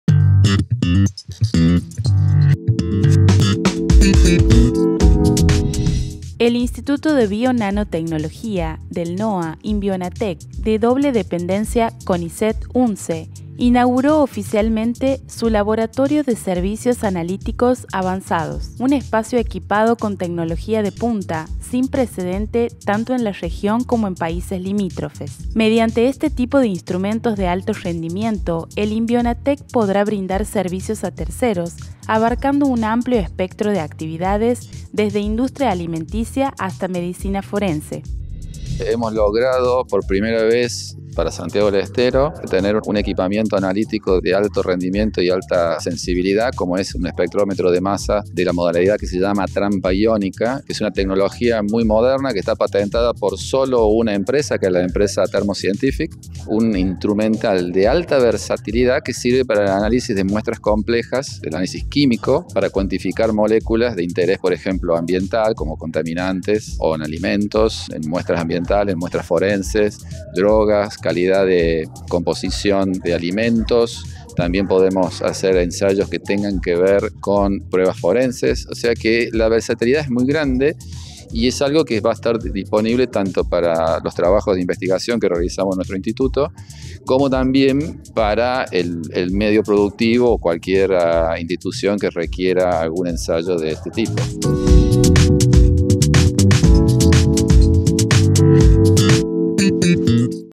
Laboratorio con servicios únicos en la región
🔹 El Instituto de Bionanotecnología del NOA inauguró su Laboratorio de Servicios Analíticos Avanzados (LabSAA), equipado con tecnología de punta para el análisis de alimentos, agroquímicos, medicamentos, entre otras áreas de investigación y desarrollo.